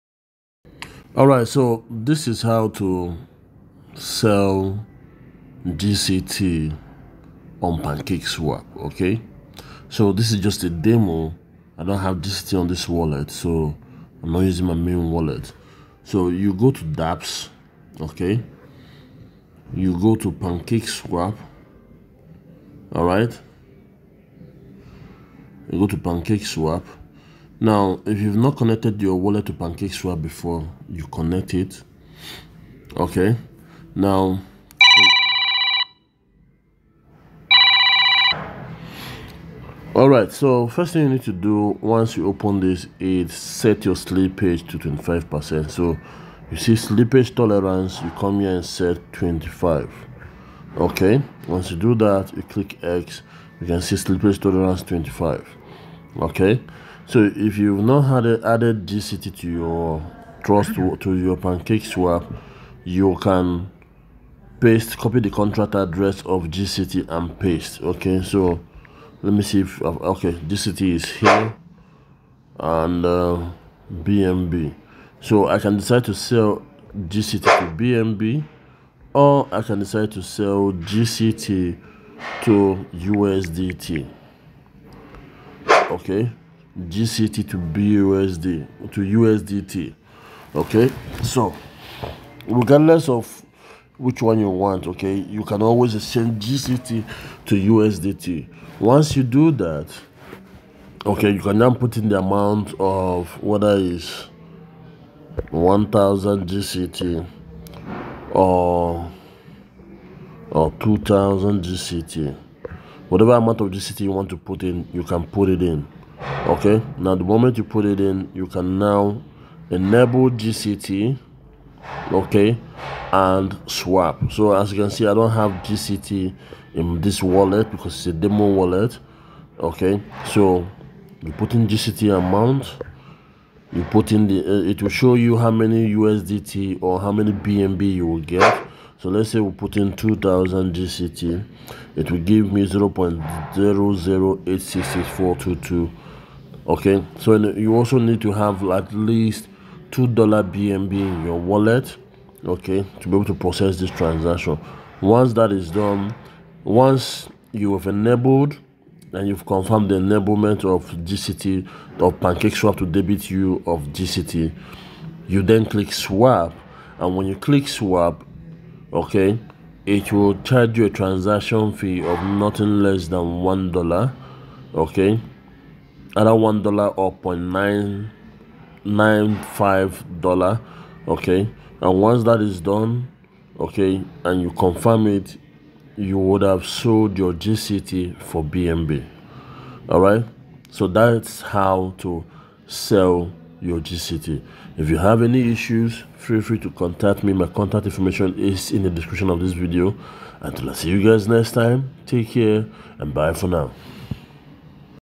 How to Trade Global Community Token (GCT) on Pancakeswap Exchange
GCT contract address:

0x72D69291f3AaA06eA484B29cfCECe47Be2764aEd

Decimal: 9

Name: Global Community Token

Network: SmartChain

To buy GCT with BNB, visit Pancakeswap Exchange